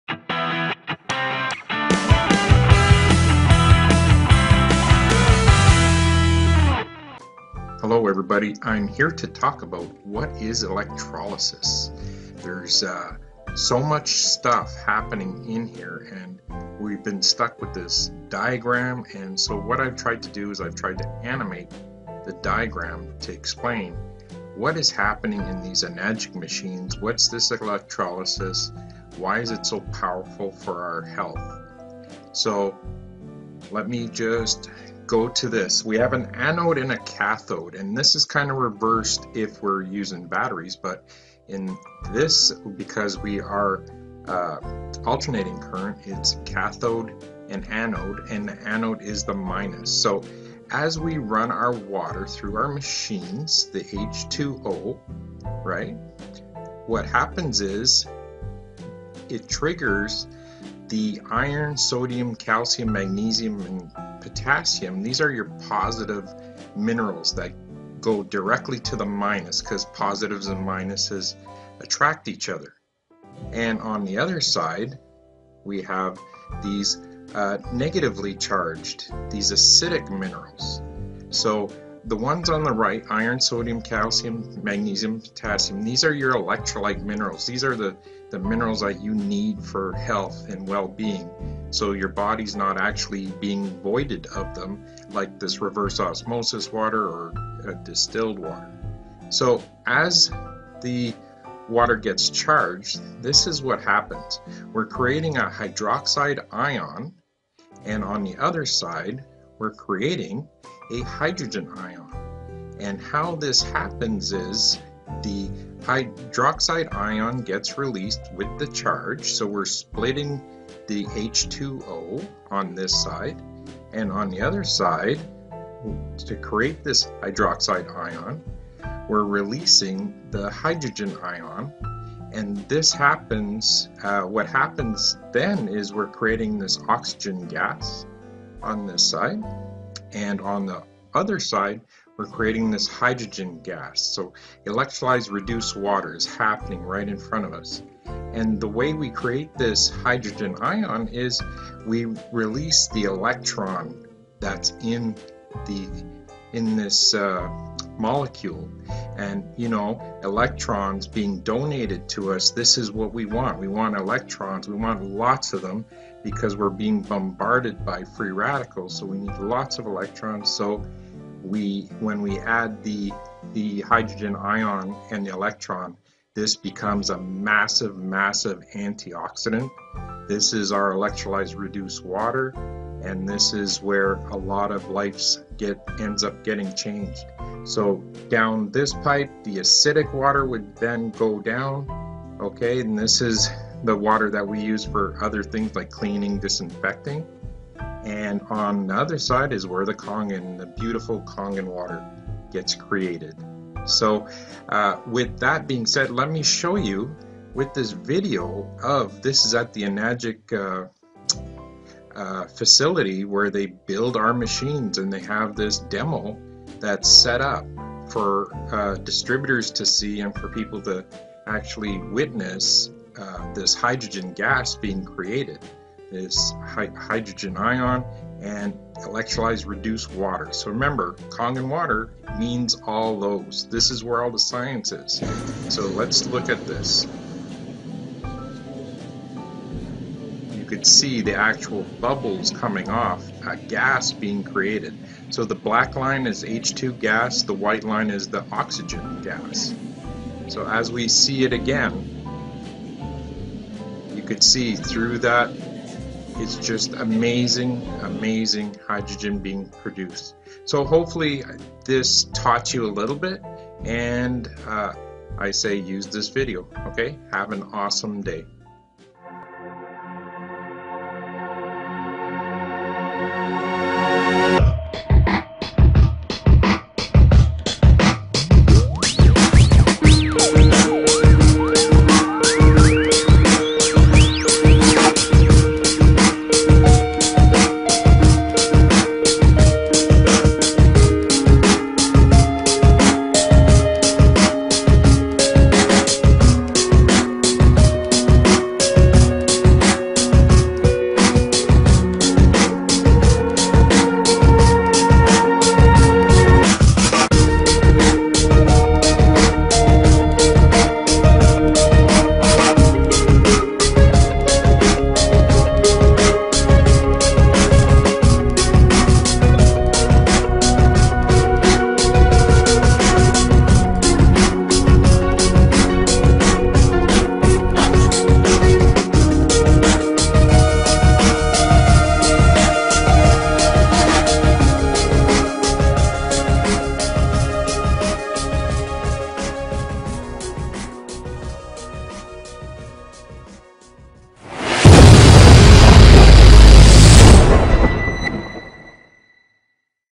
【長知識】什麼是電解？What is electrolysis?
了解什麼是電解原理？
為什麼氫對人體健康那麼重要？

「免費家居水測試」
包括：
酸鹼值
潔淨度
抗氧化力
洗農藥能力
去油力
殺菌力
*預約請找您的介紹人/分銷公司